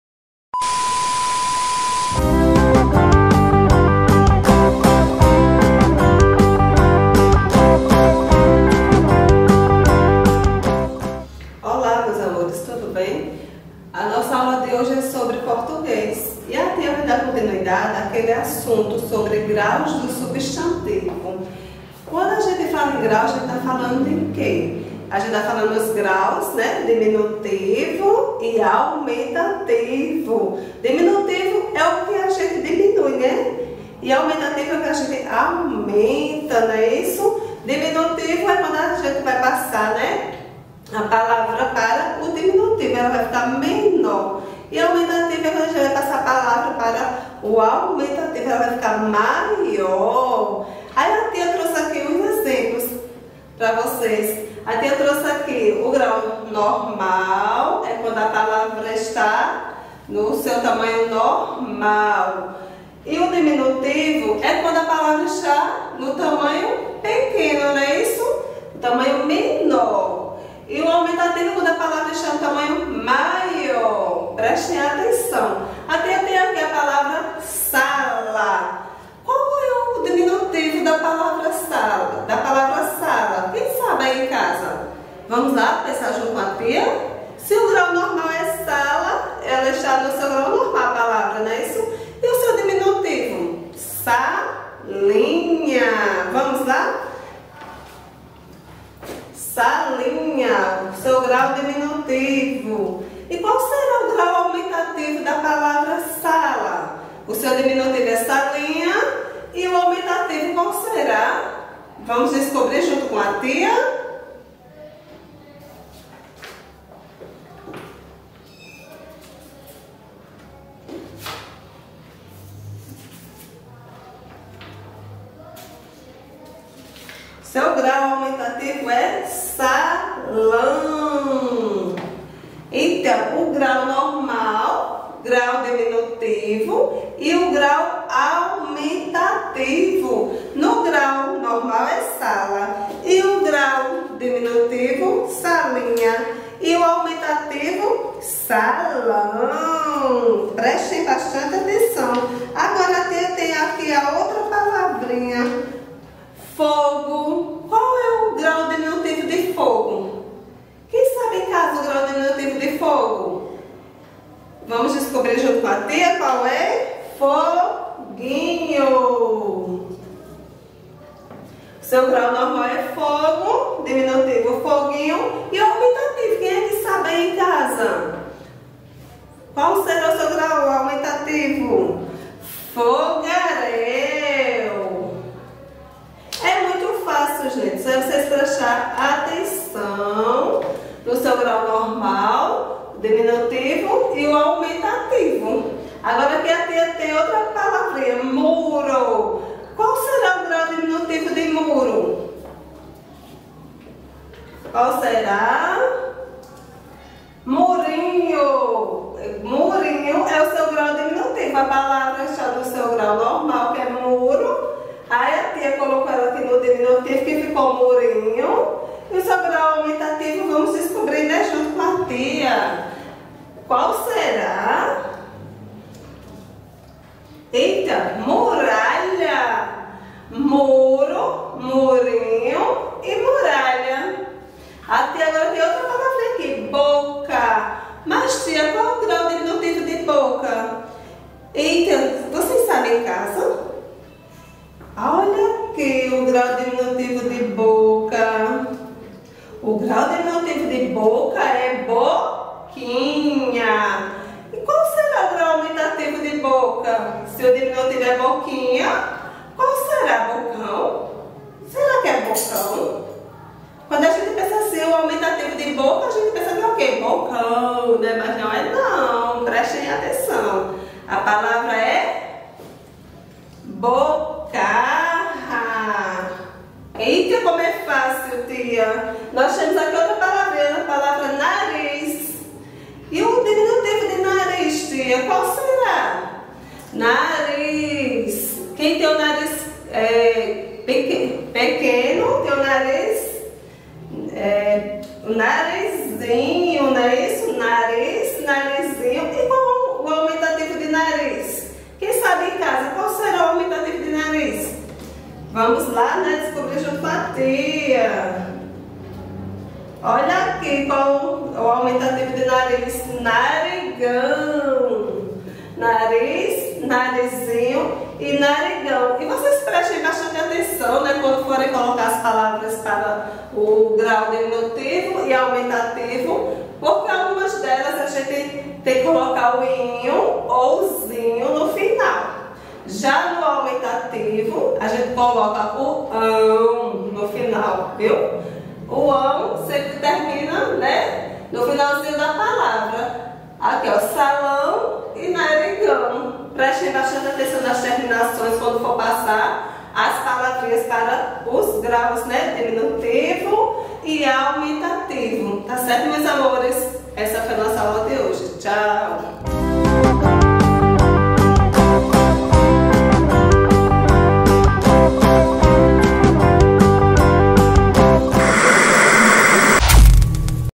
2º ANO PORTUGUÊS GRAUS DO SUBSTANTIVOS AULA PARA 30082021
2º ANO PORTUGUÊS GRAUS DO SUBSTANTIVOS AULA PARA 30082021 /// Aula sobre  a flexibilização do Substantivos de acordo com seu Grau;